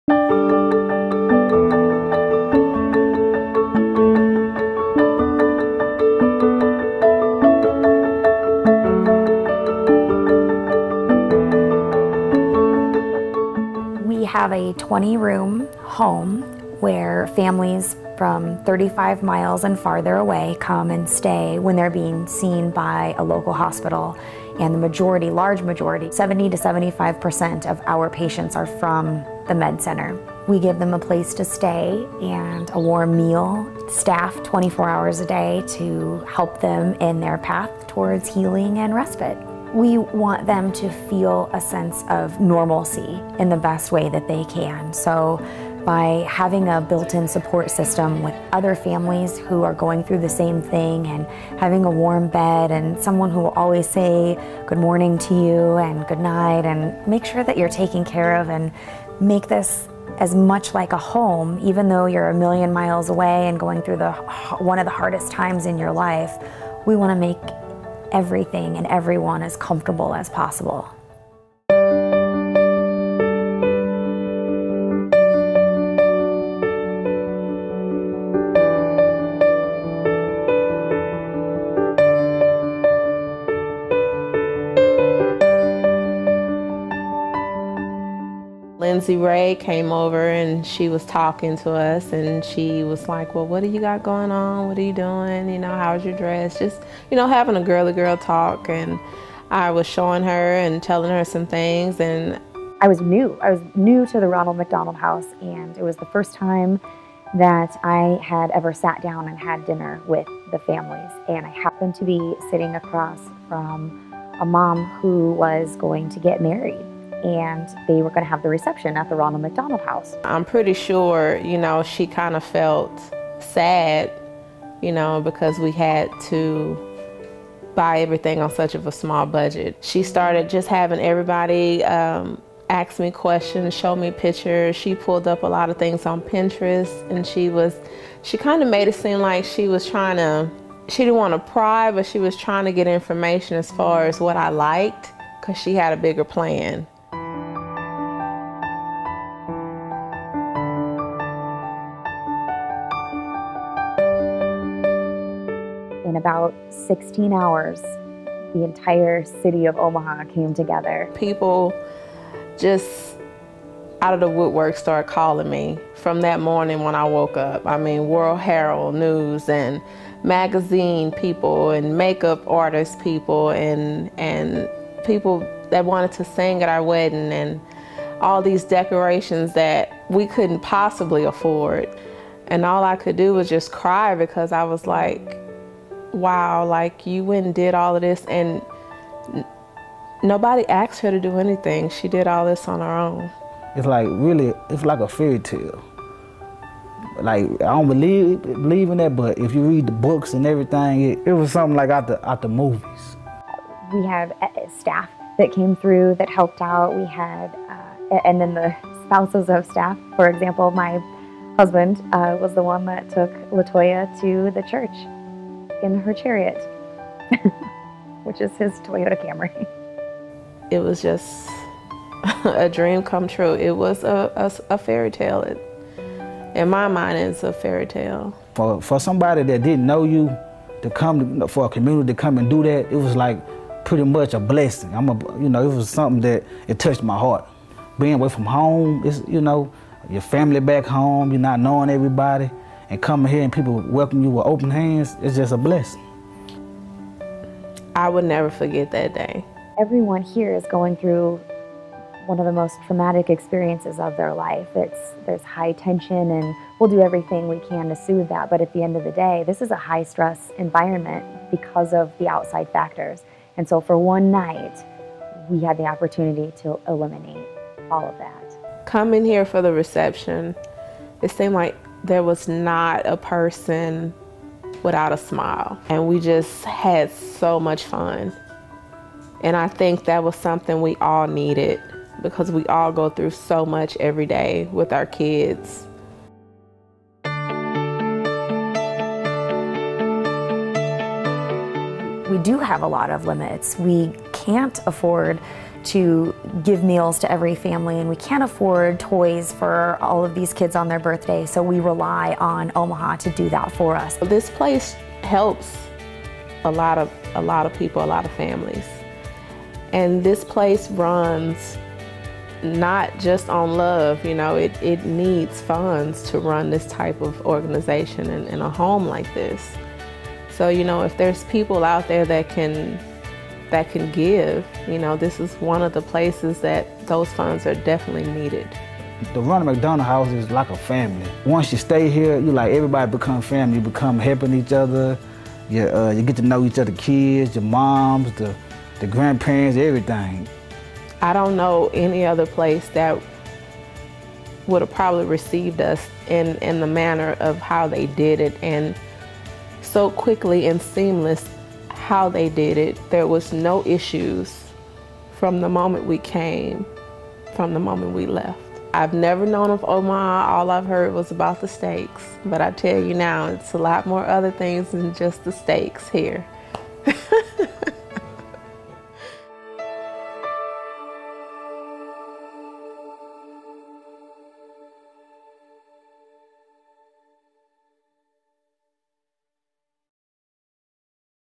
The Ronald McDonald House Charities In Omaha - The Nebraska Medical Center
This couple stayed at The Ronald McDonald House in Omaha for 504 days while their son was being treated at The Nebraska Medical Center. After hearing of their plans to get married, one RMH employee took it upon herself to make sure their wedding day was one they would never forget. With only about 16 hours to plan things, the entire Omaha community came together to give them a fairytale wedding and reception, which was hosted at the local Ronald McDonald House.

Through this effort, this video shows the vital role the Ronald McDonald House plays in the lives of its residents, along with the great need for outside funding to make events like these a possibility.

It also touches on the relationship between the Ronald McDonald House and The Nebraska Medical Center, which accounts for nearly 75 percent of the facility's residents.